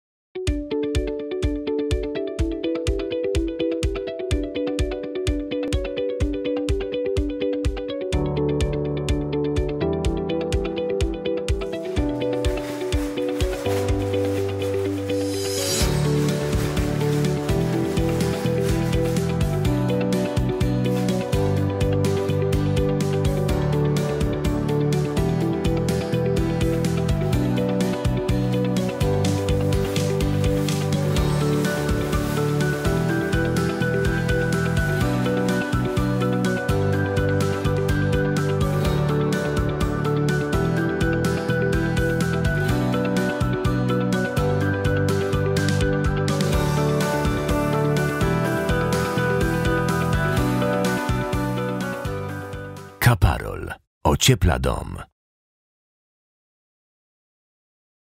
Bezpyłowa masa klejowo-szpachlowa X-TRA 300 DUST FREE
Mineralna sucha zaprawa do wykonywania warstw zbrojonych w systemach ociepleń Capatect EPS i MW w wariancie LONGLIFE. Wzmocniona włóknem węglowym i odporna na uderzenia.